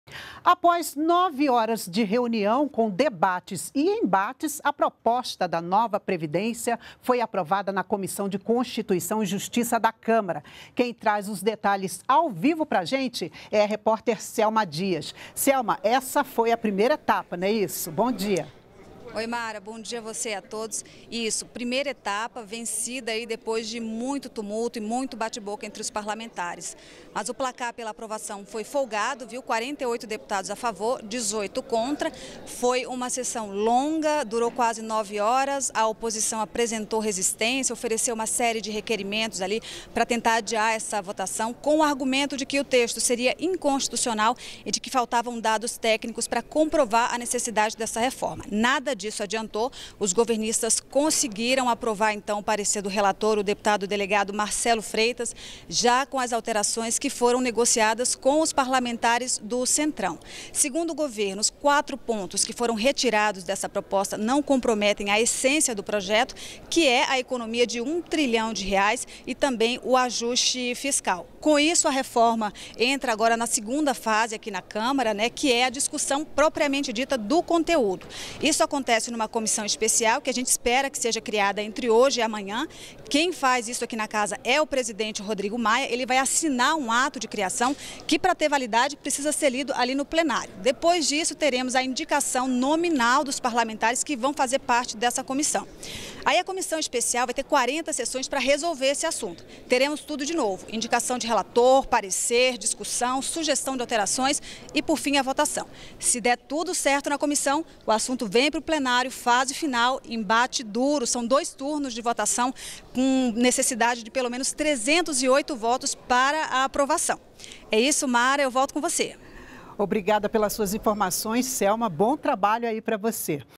Proposta da Nova Previdência é aprovada em comissão da Câmara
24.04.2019 - A proposta da Nova Previdência foi aprovada na Comissão de Constituição e Justiça na Câmara. Esta foi a primeira etapa aprovada, após 9 horas de debate. Veja com mais detalhes.